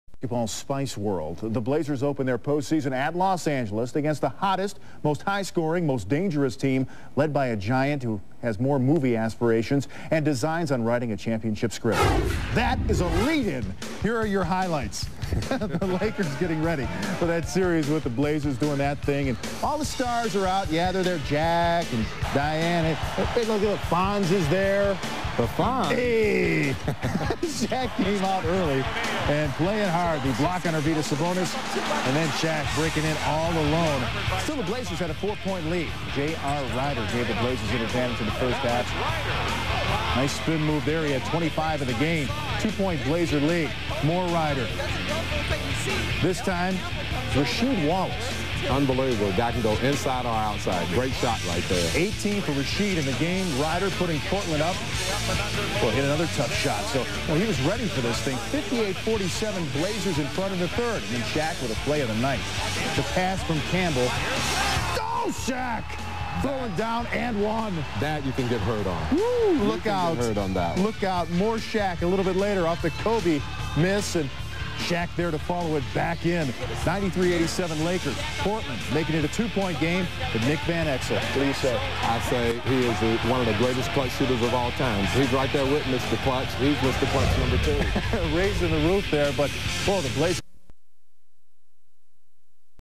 1998: Shaq Dunks All Over Kelvin Cato
One of the best dunks ever by Shaq